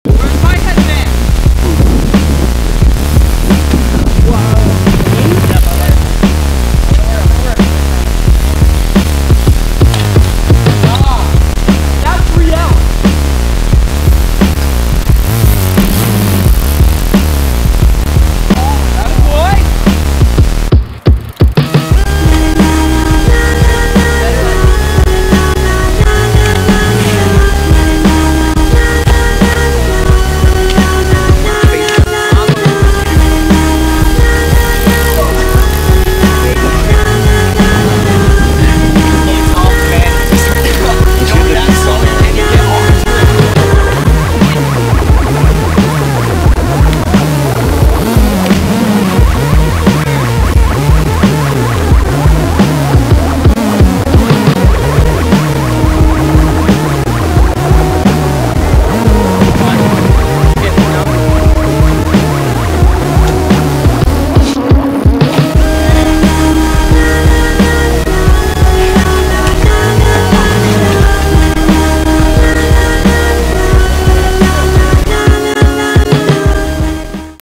Study Hall Bike Sesh
Riding around WHS during study hall on the last day of school (before finals).